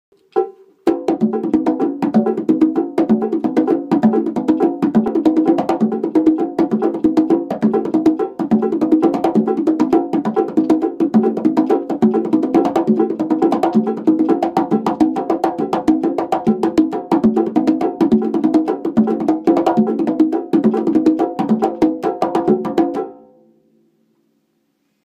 CONGAS - BASE PARA RUMBA ABIERTA PARA 3 CONGAS
DAVID SANTOS PEREZ, profesor en Taller de Percusion Alicante (clases particulares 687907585).Tocando base de rumba abierta para tres congas.